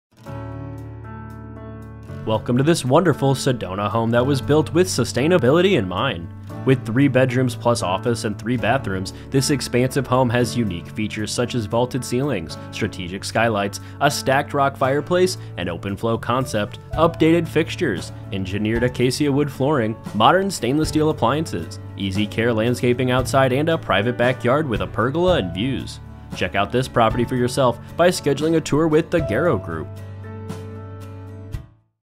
110 Sugarloaf Road Sedona, AZ | ColdwellBankerHomes.com
110 Sugarloaf Road, Sedona, AZ

This wonderful Sedona home was built with sustainability in mind! Lovely Passive Solar design sternly constructed to pass the test of time. With three bedrooms plus office and three bathrooms, this home showcases unique features such as vaulted ceilings, strategic skylights for natural light and win